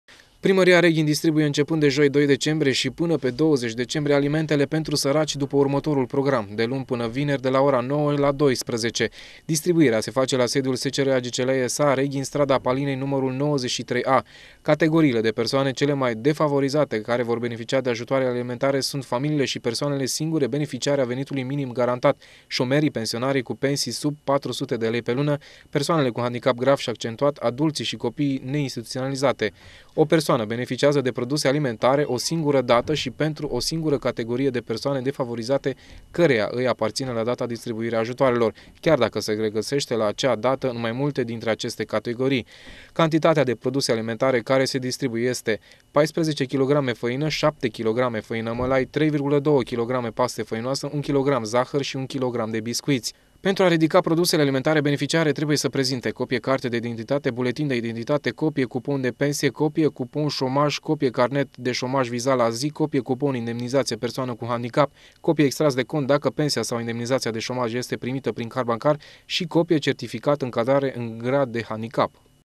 ALIMENTE PENTRU SARACI
Beneficiarii alimentelor din stocul naţional de intervenţie îşi pot ridica produsele începând cu 2 decembrie. Acestea se acordă persoanelor defavorizate o singură dată şi pentru o singură categorie de persoane defavorizate, chiar dacă ei se regăsesc în mai multe dintre categoriile beneficiare.